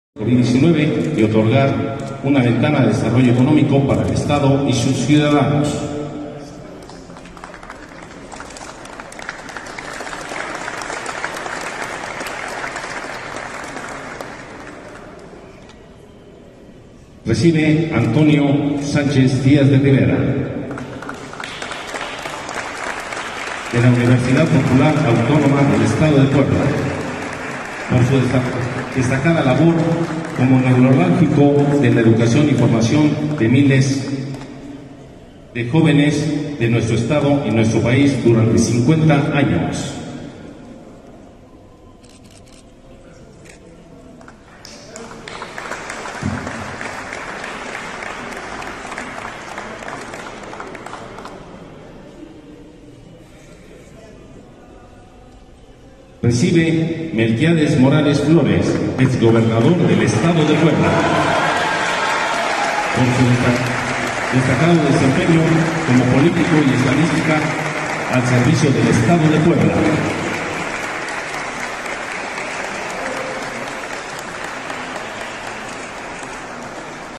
El Ayuntamiento de Puebla entrega reconocimientos Síntesis a poblanos destacados
Desde las instalaciones del Palacio Municipal, el Ayuntamiento de Puebla
entrega reconocimientos Síntesis a poblanos destacados, entre ellos el exgobernador Melquiades Morales y el secretario de Salud estatal, José Antonio Martínez.